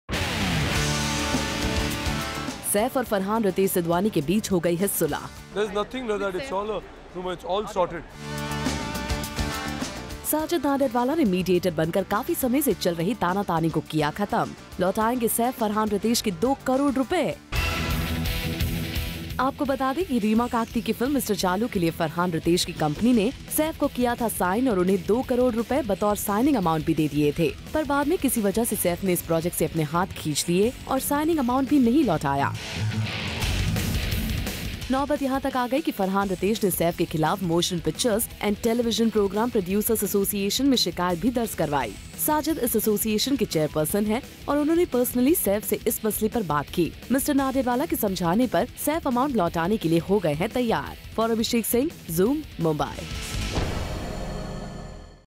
Sajid Nadiadwala Plays Mediator For Saif Ali Khan | Bollywood News
Someone always helps other is  Bollywood and this time
Sajid Nadiadwala plays mediator for Saif Ali Khan.

Turn On To The All New zoom:-

Catch the latest Bollywood gossip of your favorite stars under one roof.